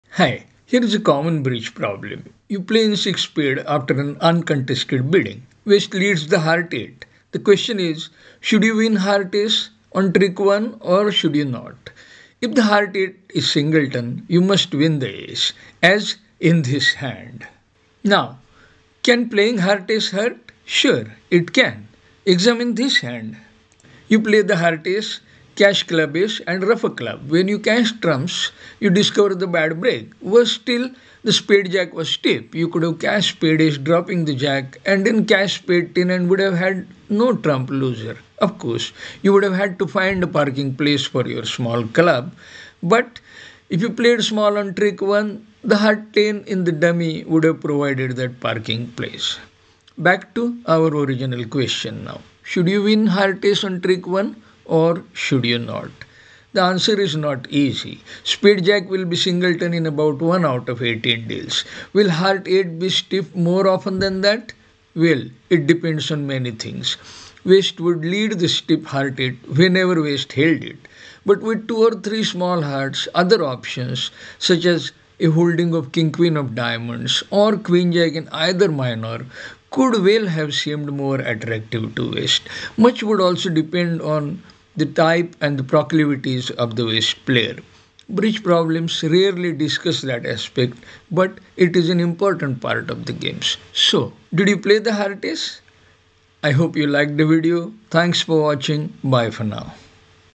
Deals : To win (Heart Ace) or not to win on trick one.
Should you win HA on trick one or should you not? I face this problem yesterday when playing a ranked game on Introbridge. The answer is not easy.
This video briefly examines the two options and the nature of the problem.

SJ will be singleton in about one out of 18 deals. Will H8 be stiff more often that that? Well, it depends on many things. West would lead the stiff H8 whenever West held it, but with two or three small Hearts, other options, such as a holding of KQ of Diamonds, or QJ in either minor could well have seemed more attractive. Much would also depend on the type and the proclivities of the West player. Bridge problems rarely discuss that aspect, but it is an important part of the game.

Playlist Reference: IBD137
Event Reference : IntoBridge Board833 played on 12FEB2024